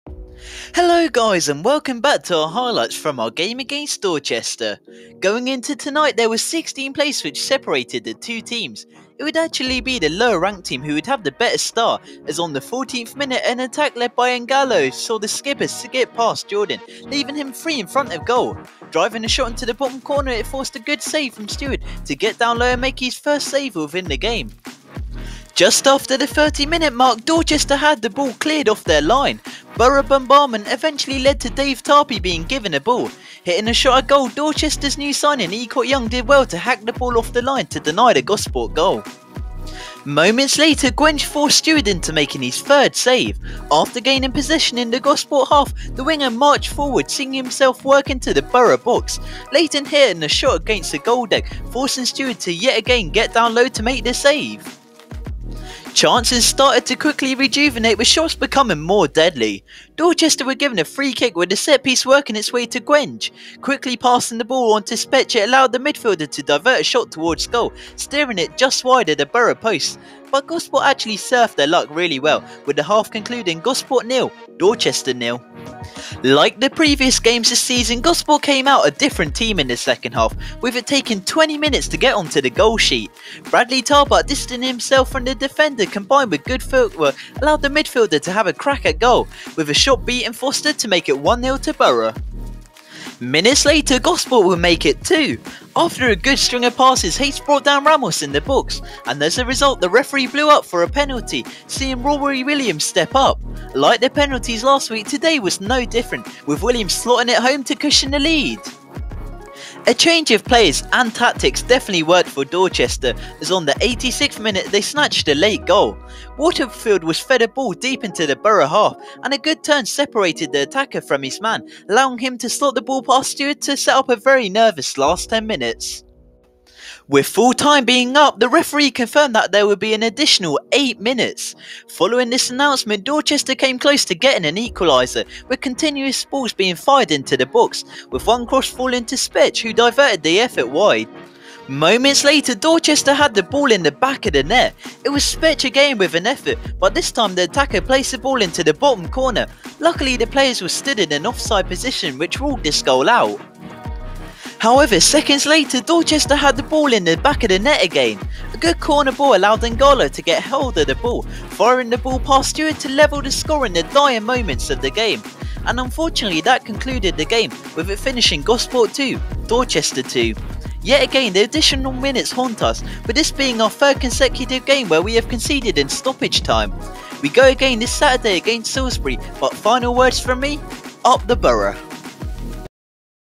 MATCH HIGHLIGHTS SPL | Gosport borough vs Dorchester Town
The Match Highlights from our game against Dorchester Town FC.

Match footage- VEO camera

Presenter- James McIntosh

Thumbnail image- Tom Phillips

Thumbnail- James McIntosh